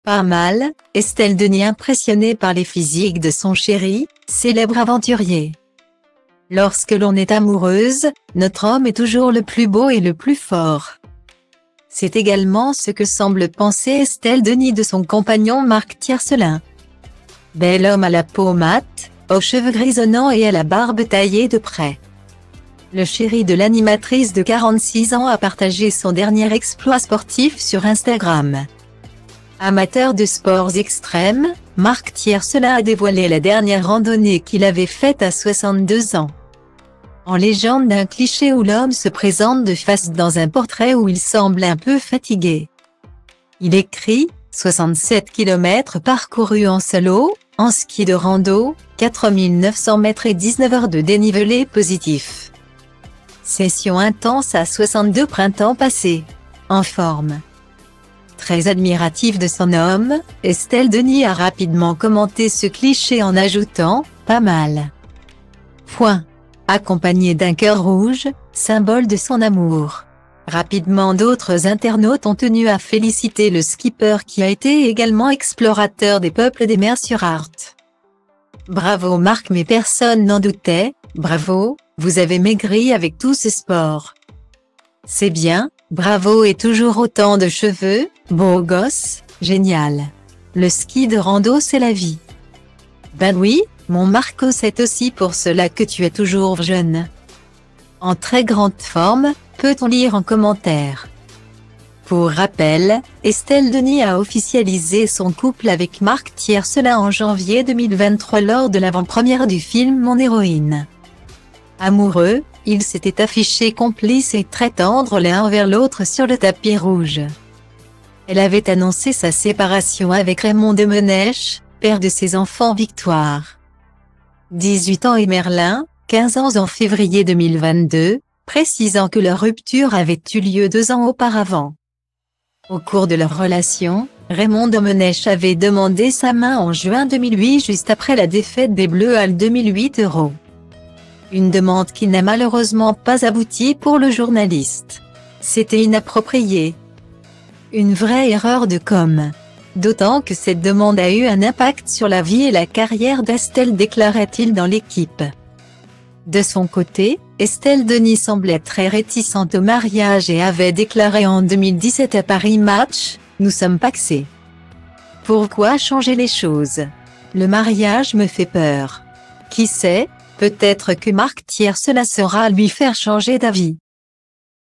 "Pas mal..." : Estelle Denis impressionnée par les physiques de son chéri, célèbre aventurier
Lorsque l'on est amoureuse, notre homme est toujours le plus beau et le plus fort.C'est également ce que semble penser Estelle Denis de son compagnon Marc Thiercelin.Bel homme à la peau mate, aux cheveux grisonnants et à la barbe taillée de près,thank for watching my video your sub my channel